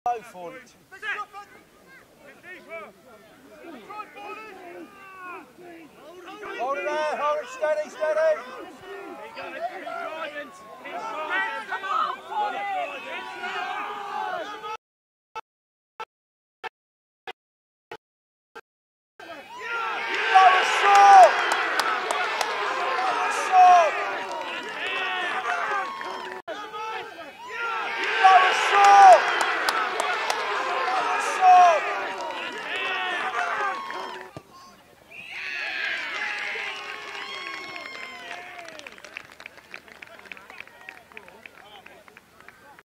Lymington v Fawley 22/4/23 Clip 6
Fawley loss 28 - 26. Try & conversion Lymington.